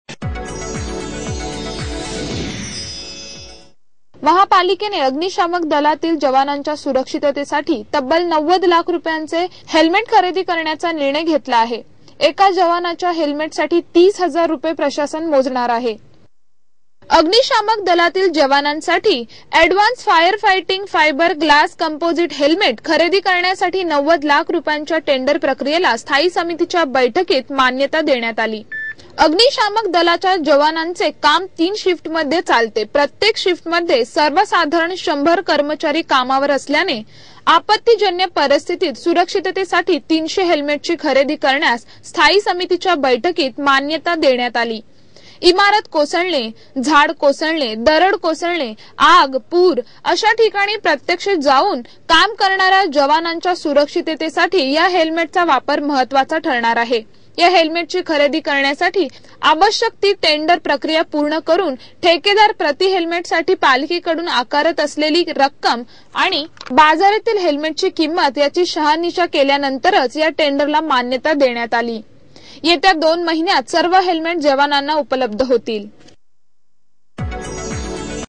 अग्निशामक दलाच्या जवानांच्या सुरक्षेसाठी हेल्मेट खरेदी
अग्निशामक दलाच्या जवानांच्या सुरक्षेसाठी हेल्मेट खरेदी; ९० लाख रुपायांच्या टेंडरला 'स्थायी'ची मान्यता